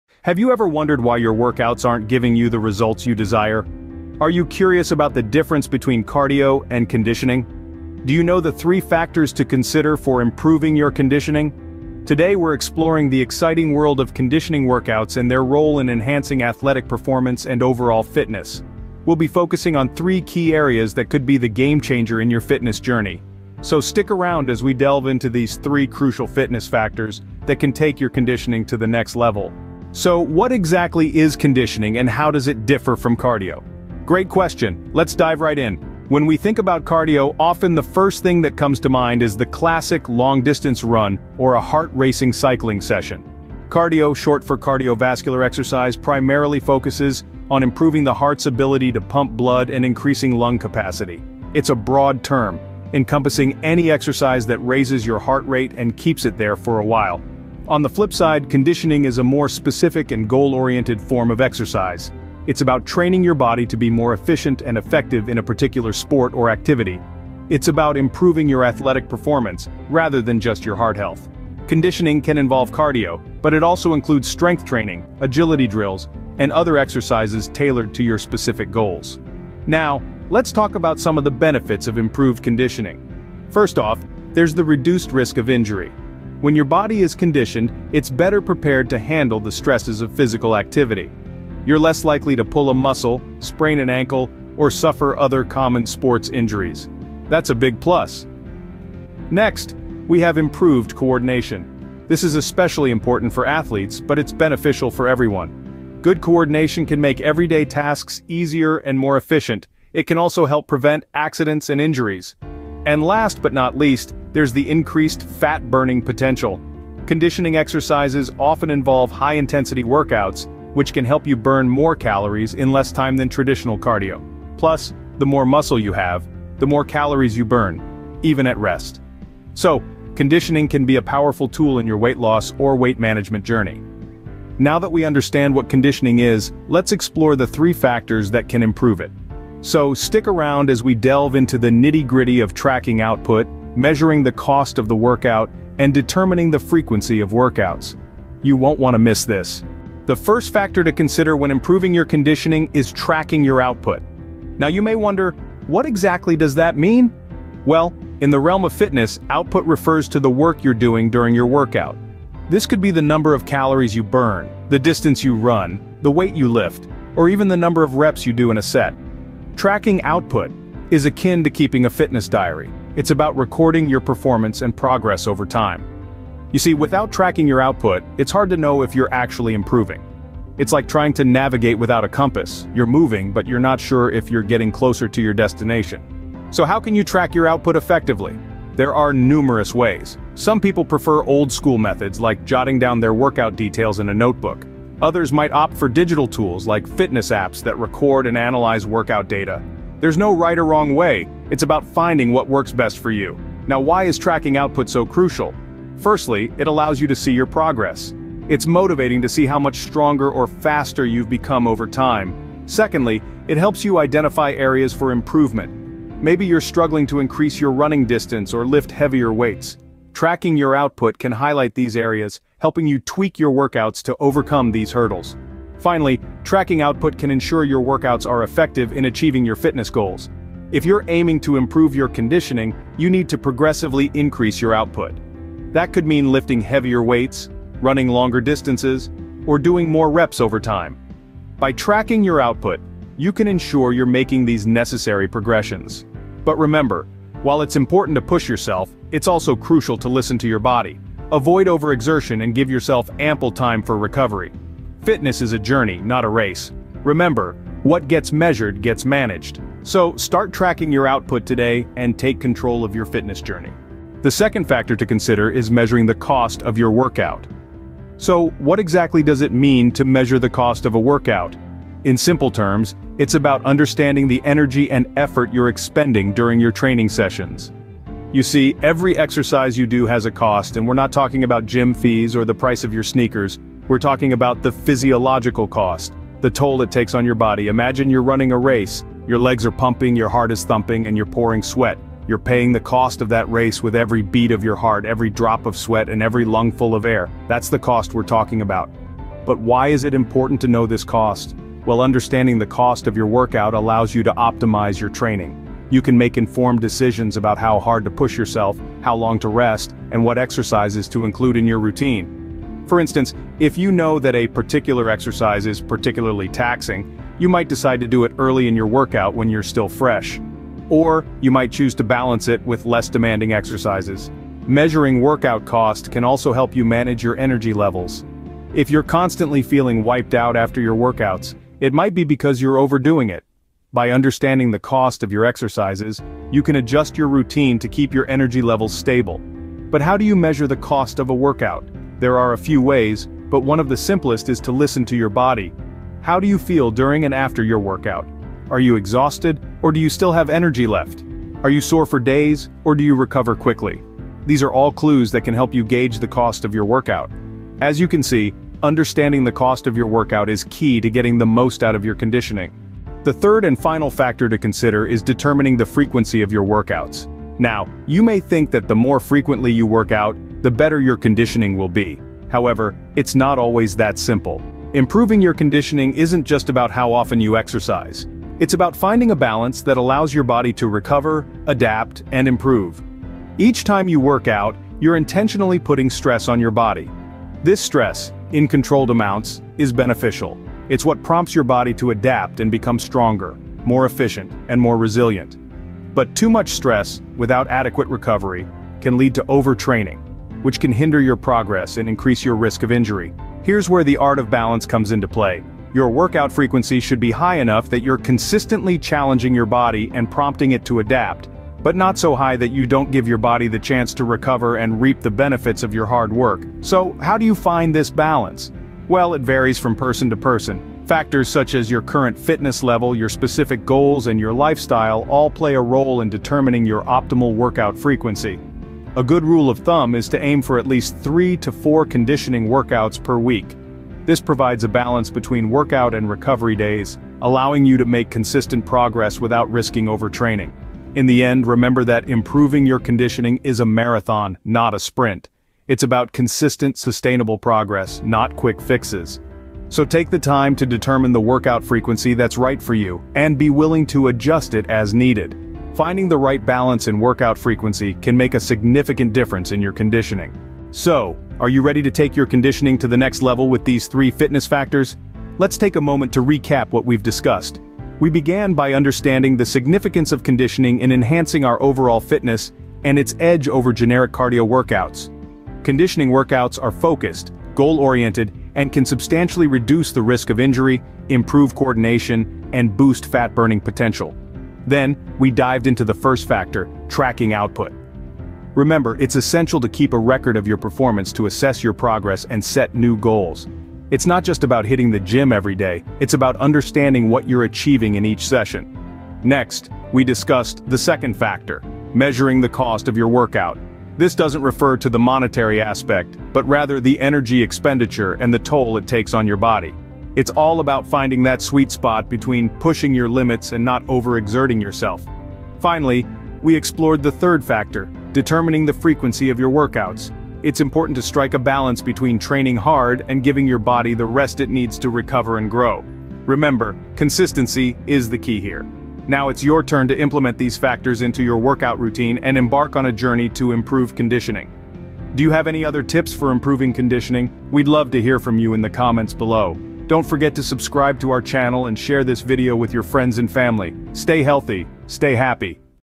Boost Your Conditioning: 3 Fitness Factors to Consider!
In this video, we're going to discuss three important fitness factors to consider when trying to boost your conditioning. By understanding these factors, you can create a powerful conditioning routine that will help you reach your fitness goals!

If you're looking to boost your conditioning, then this video is for you! By understanding the effects of these three factors, you can create a conditioning routine that will help you reach your fitness goals!

Whether you're a fitness enthusiast, a beginner, or just looking to improve your overall well-being, we've got you covered.

😁 Remember, your health is your wealth, and we're here to support you every step of the way. Let's make positive changes together and live our best lives!

Chapters:
00:00:00 Introduction
00:00:29 Understanding Conditioning
00:02:29 Factor 1 - Tracking Output
00:04:23 Factor 2 - Measuring Workout Cost
00:06:02 Factor 3 - Determining Workout Frequency
00:07:40 Conclusion